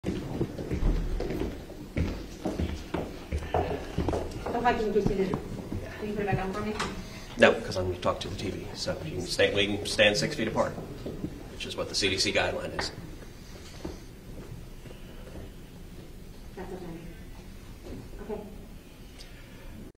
Ted Cruz refuses to wear mask outside hearing room
Before speaking to reporters, Senator Ted Cruz removed his mask outside the hearing room for President Donald Trump's U.S. Supreme Court choice Amy Coney Barrett on Thursday.